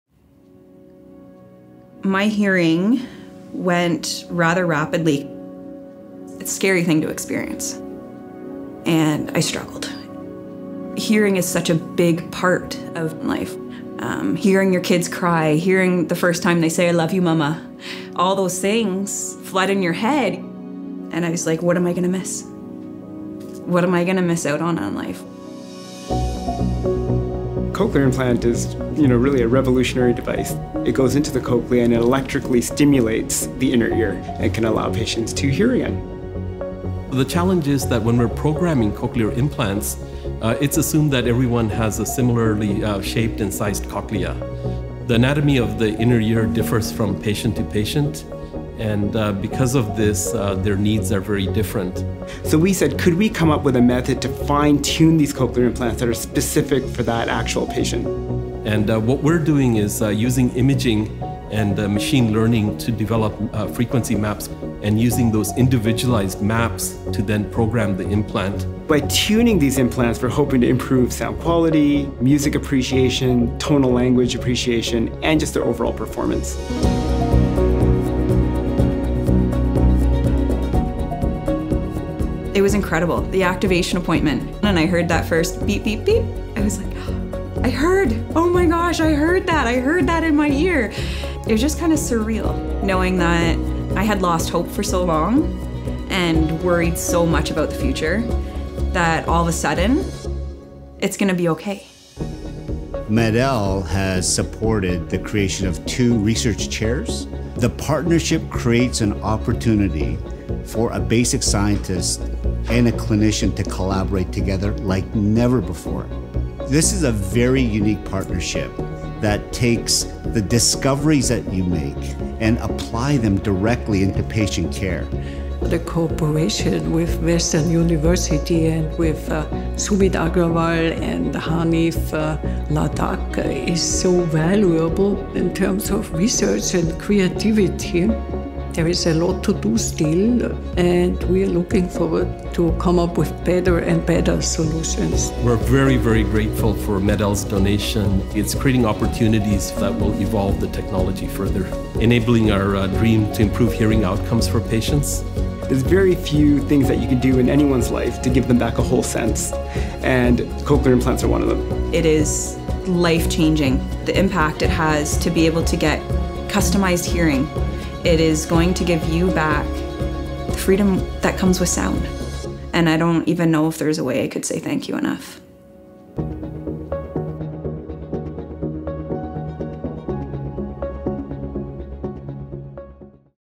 Freedom That Comes With Sound
A donation from global hearing implant company MED-EL will help patients by bolstering auditory research and innovation at Western University.

Western matched MED-EL’s gift to establish two new endowed research chairs – one in Neurotology and Translational Hearing Innovation and one in Auditory Biophysics and Engineering – to independently pursue innovative hearing technology research.

Uploaded by: Western Communications (DL)